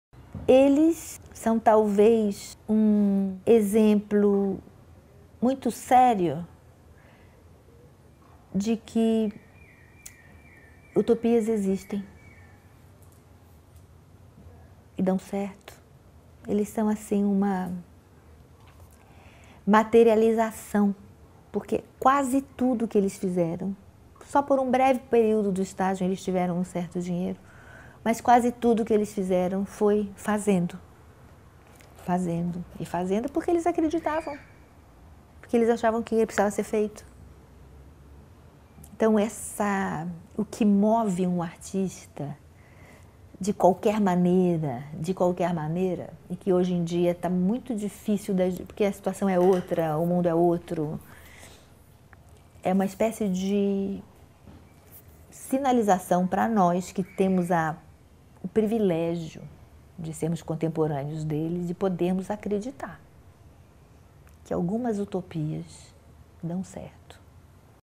Helena Katz - Utopia - Ocupação Ballet Stagium (2011)
Helena Katz é crítica de dança e professora. Acompanhou o percurso do Stagium ao longo de toda a sua história. Artigos sobre o grupo -- e sobre a dança nacional em geral -- podem ser lidos em seu